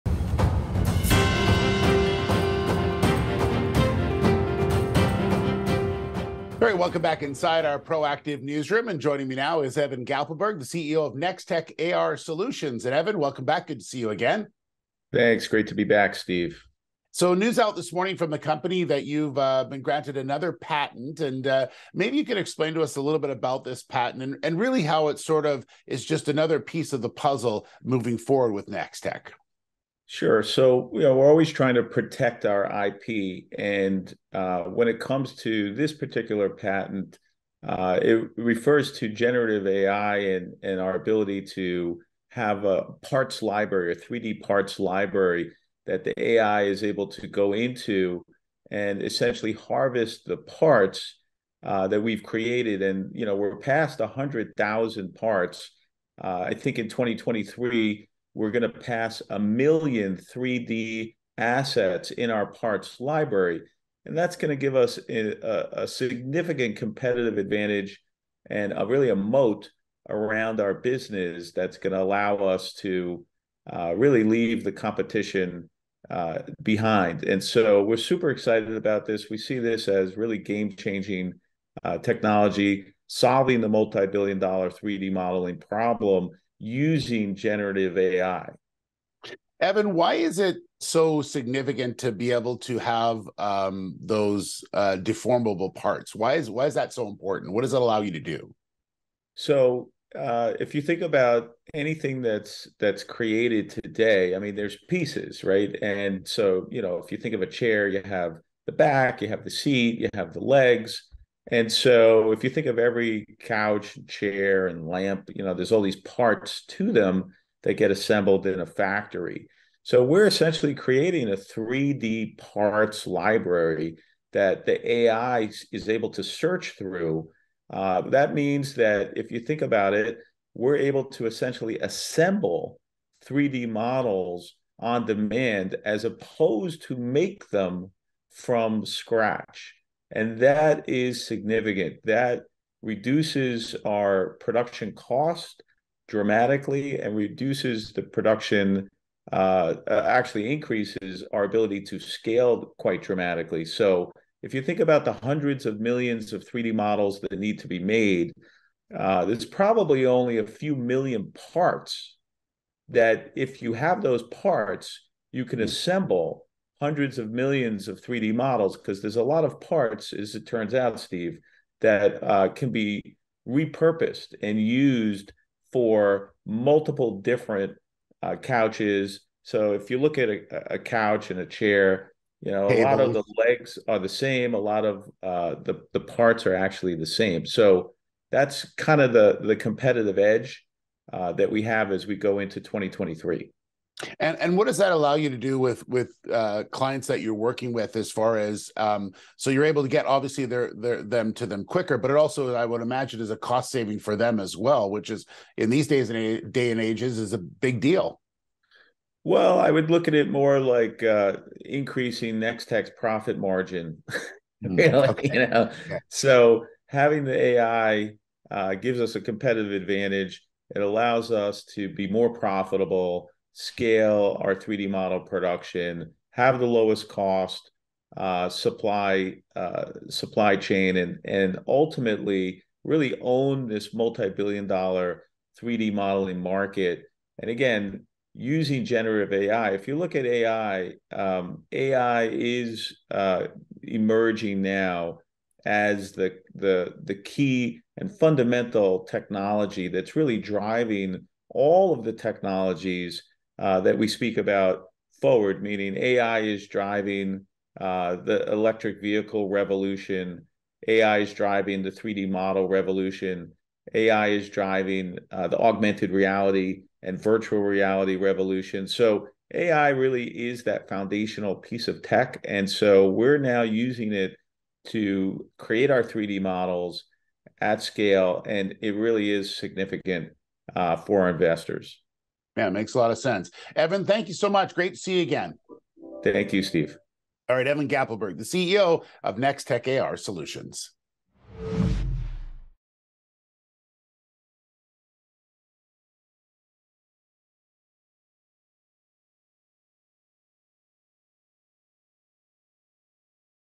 NexTech AR Solutions receives patent protection on artificial intelligence 3D model creation
NexTech AR Solutions CEO Evan Gappelberg joined Steve Darling from Proactive to share news the company has been granted a patent for its artificial intelligence technology 3D model creation.

Gappelberg telling Proactive the company is now scaling up its 3D business with expanding its library of 3D model parts past 100,000 by using through composition of deformable parts. That, according to the company, make it easier and faster to create these models.